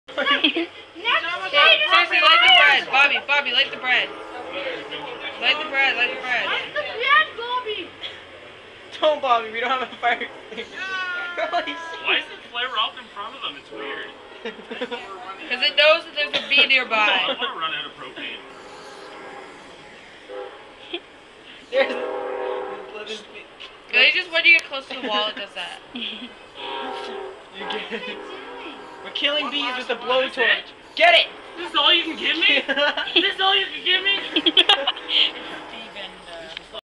bee burning
bees are getting inside my house it's time to have fun with a blow torch